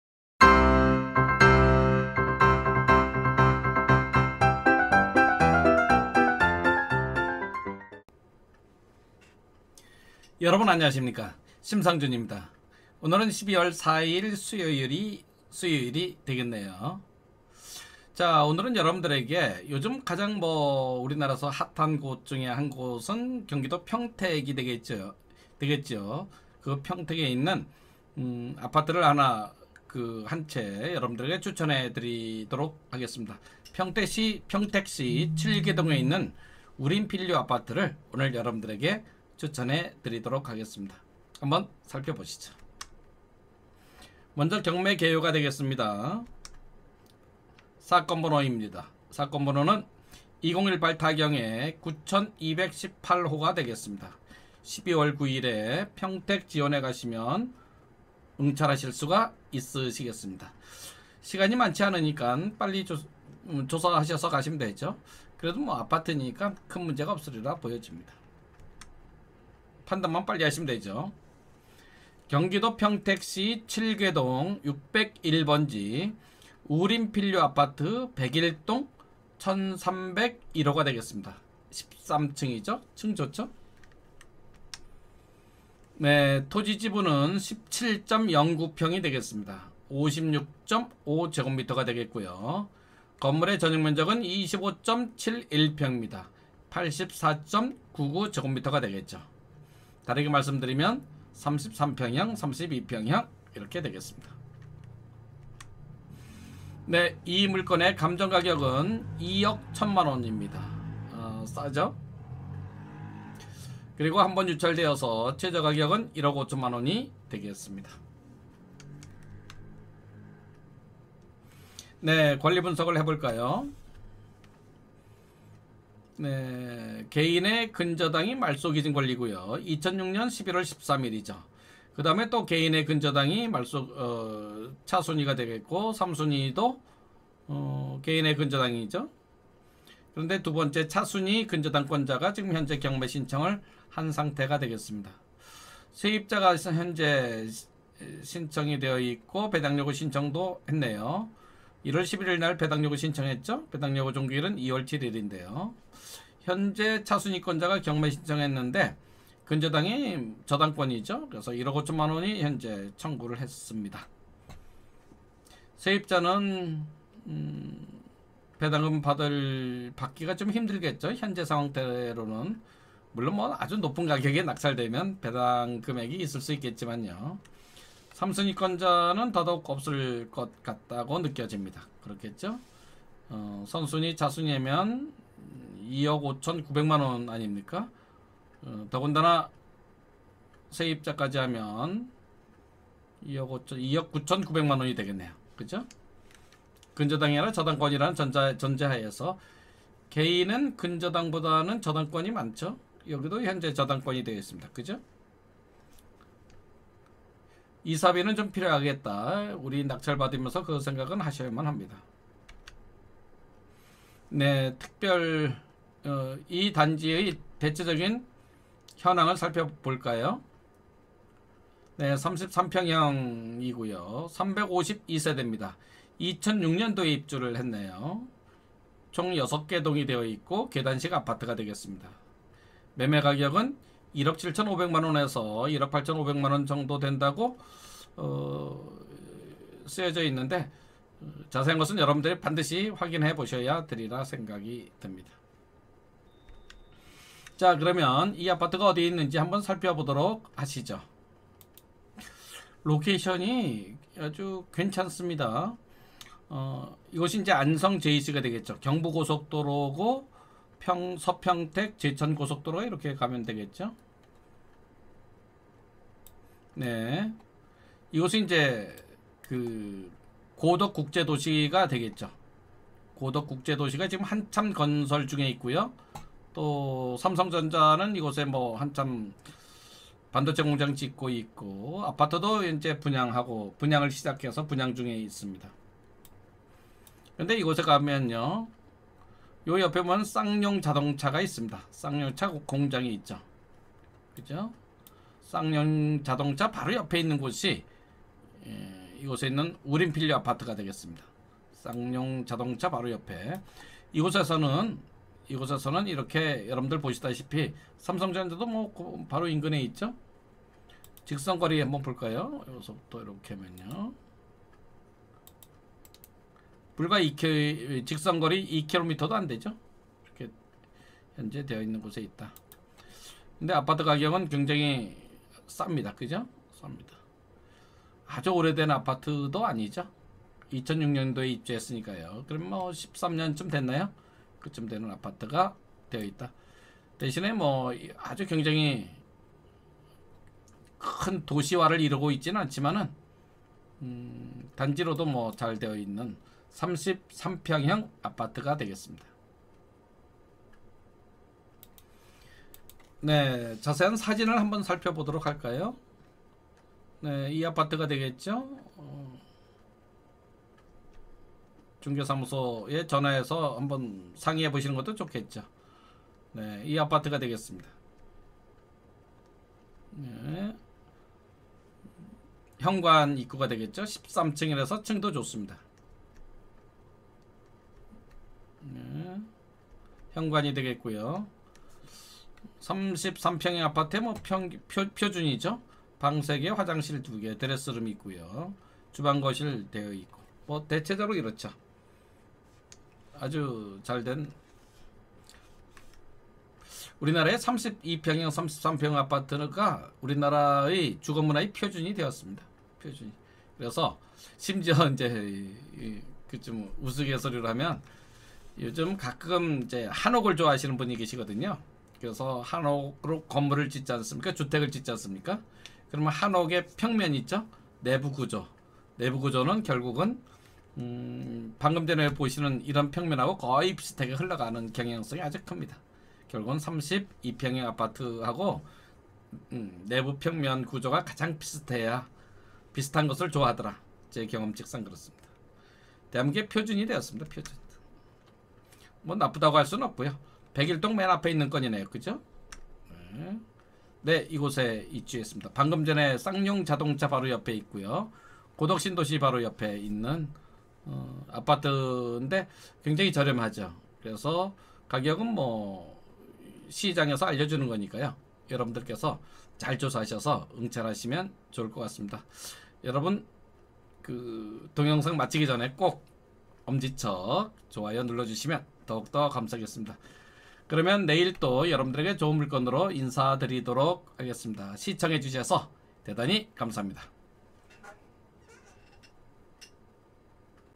[No.260 추천경매물건]평택쌍용차옆우림필유아파트32평형/1.47억(2019.12.4.)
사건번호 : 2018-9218